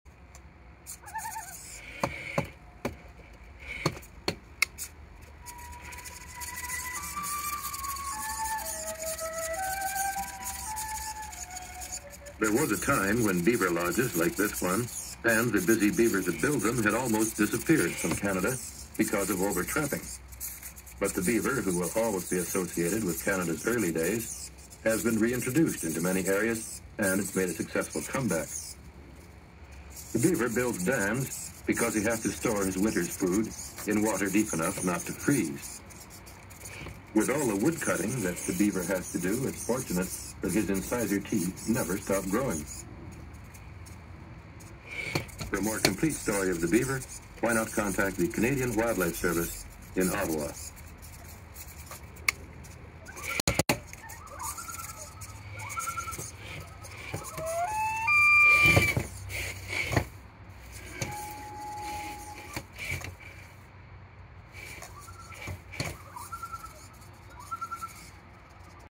Robot Loon gives a short PSA about the Beaver.
We at the Canadian Robot Wildlife Service strive to create electric wildlife and the support systems by which they function with a particular focus on low velocity migratory animals.

Mr Loon was injured after flying too close to the CN tower while the CBC transmitter was backing up the NFB archive. After the addition of some robot prosthetics, he is now rehabilitated and ready to explore the wilds of Canada once more.

Thank you to the Canadian Wildlife Service for etching these facts into my, and the Loons brain forever.